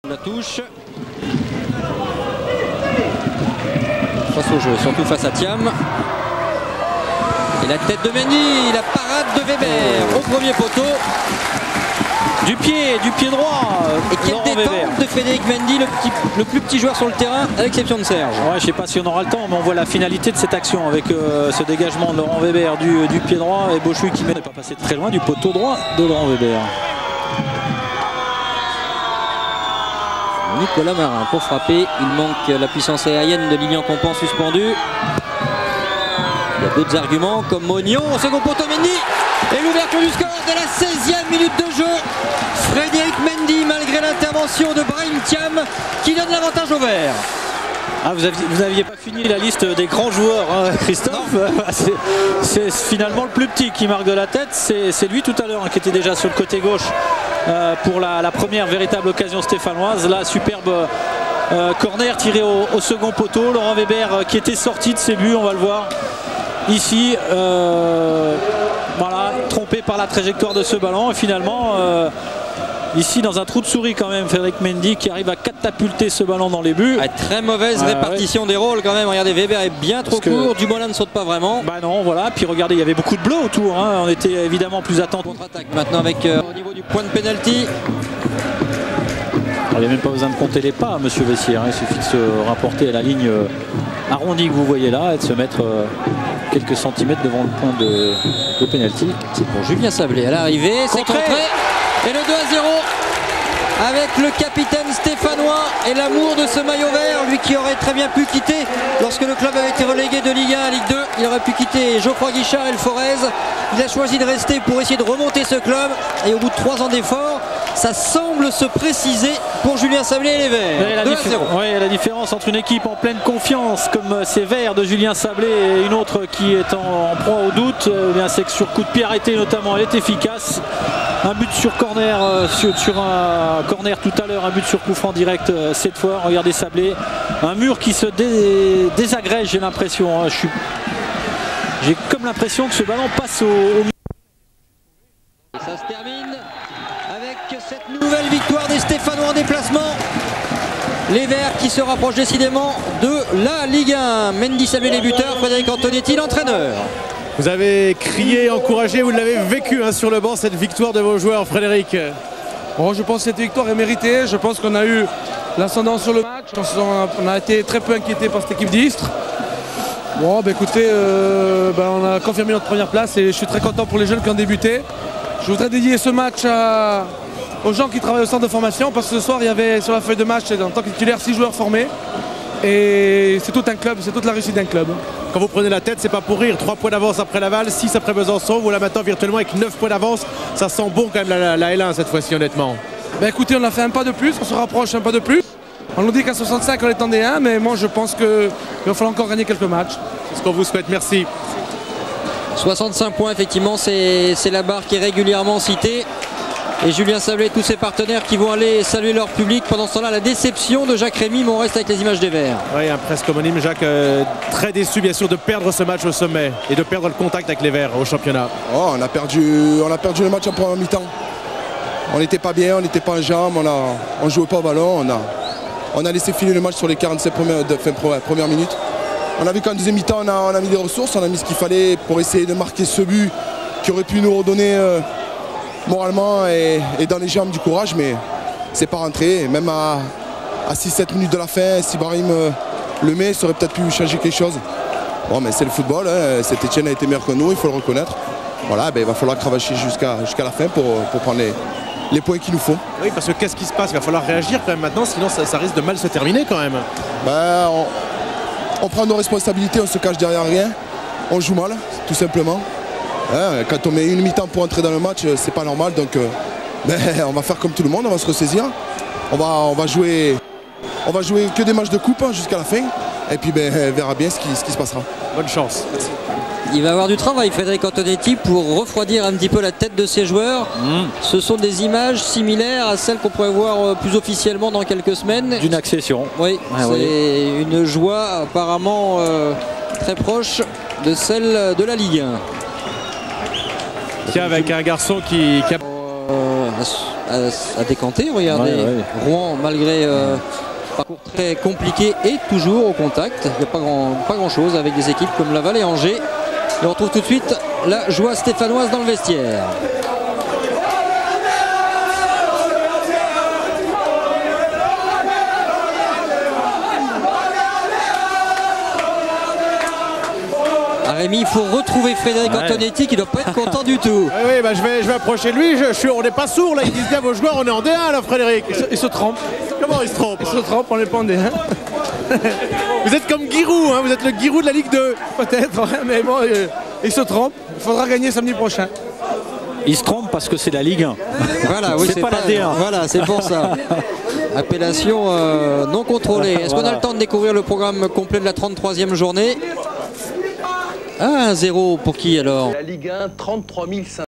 Istres 0-2 ASSE - 33e journée de L2 2003-2004
Buts : Frédéric Mendy 16', Julien Sablé 51'. L'ASSE est 1re et va remonter en L1 quelques semaines plus tard.

Images Eurosport